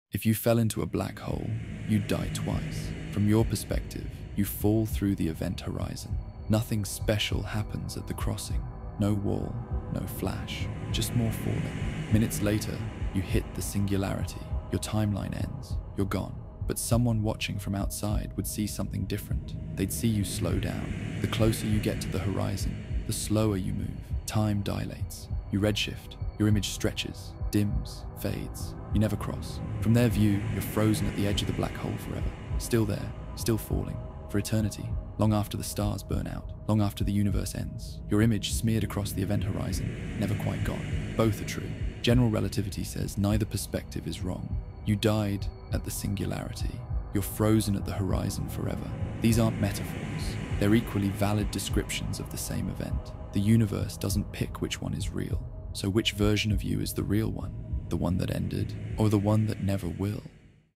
What If You Die TWICE in a Black Hole?
If you fell into a black hole, you'd die twice. From your perspective: you cross the event horizon, fall for minutes, hit the singularity. Dead. But from outside: you slow down, redshift, fade, and freeze at the edge forever. You never cross. Both are true. General relativity says neither perspective is wrong. You ended at the singularity. You're frozen at the horizon for eternity. The universe doesn't pick which one is real. So which version of you is the real one?